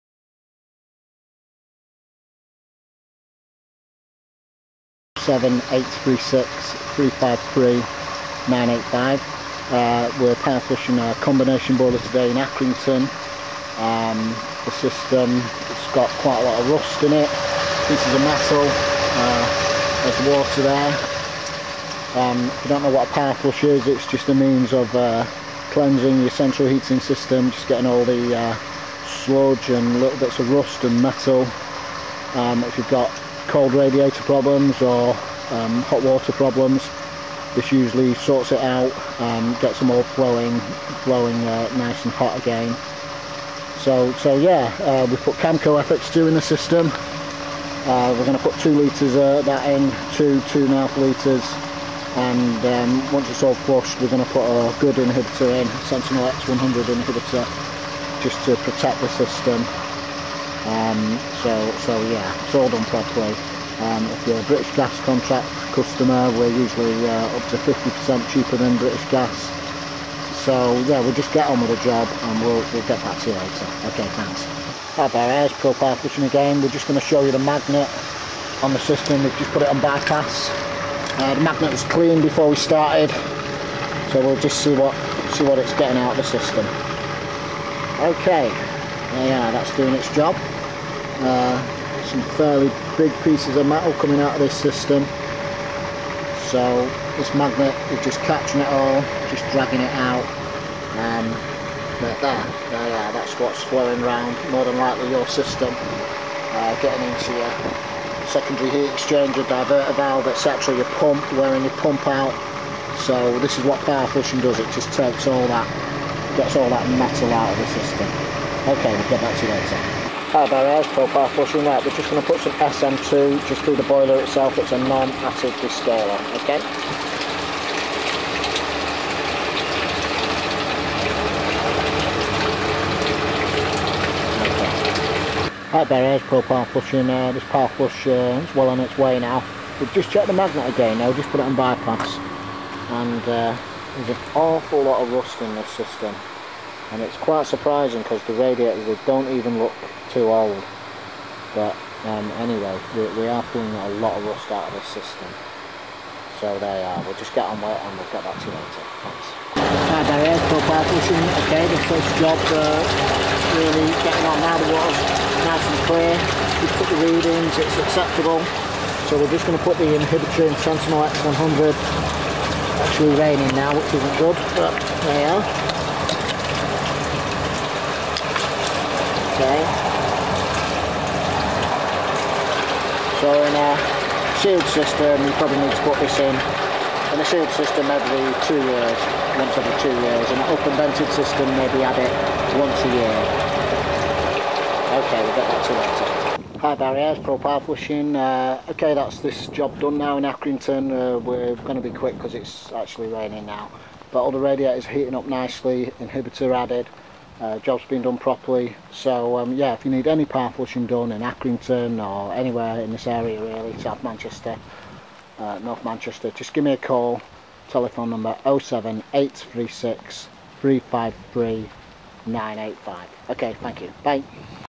Power flushing accrington video
Power Flushing accrington video of a combination boiler central heating system wich needed power flushing,We only use the best chemicals and inhibitors for our work so call me on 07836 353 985 or visit website for details.